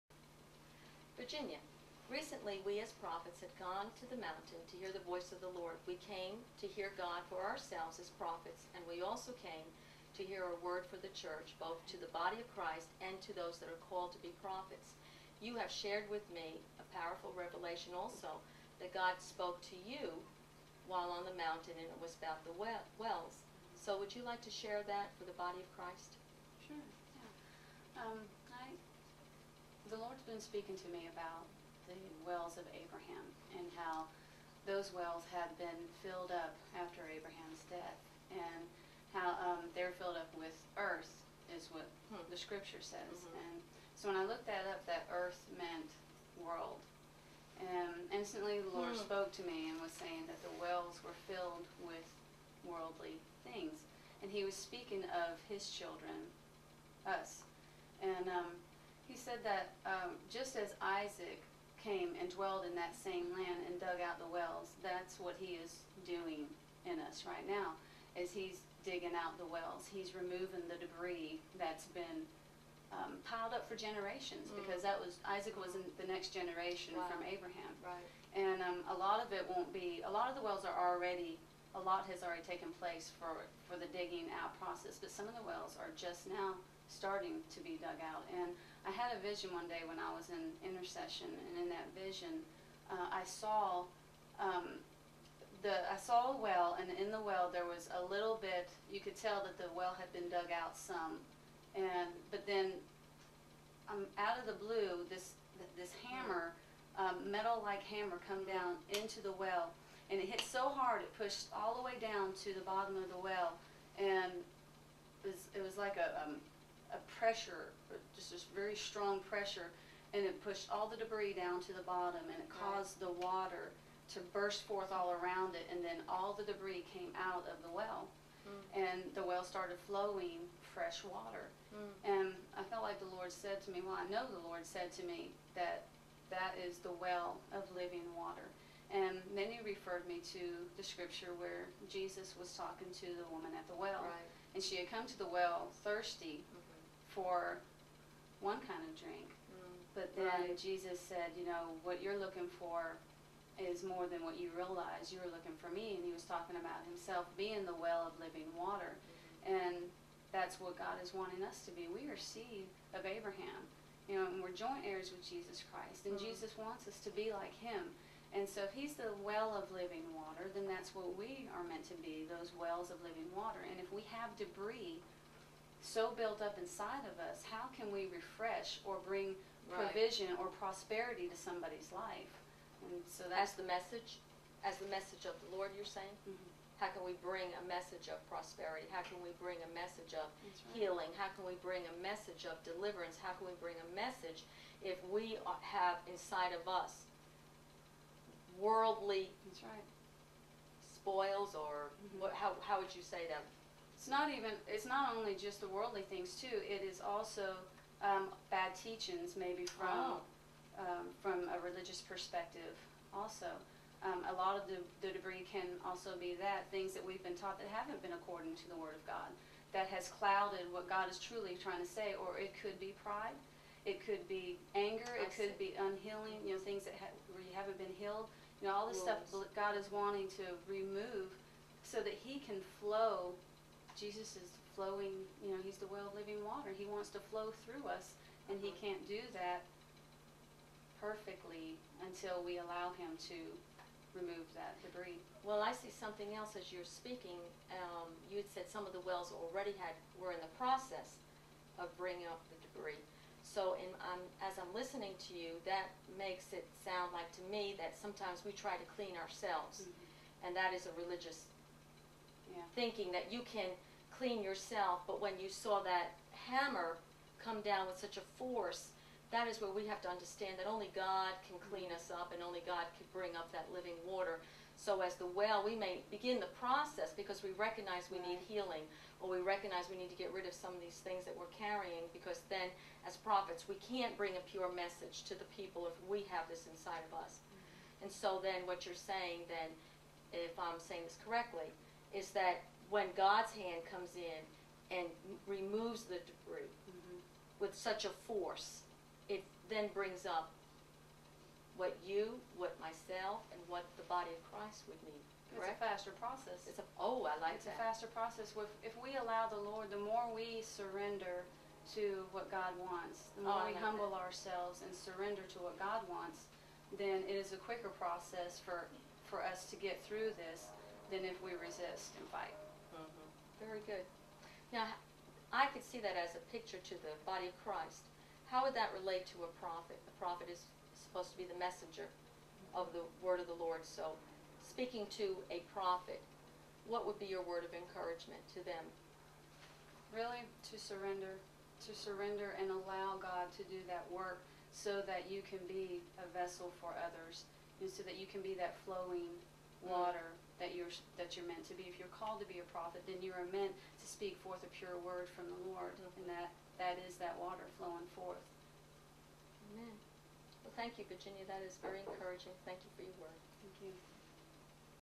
Virginia
Virginia speaks about the wells of Abraham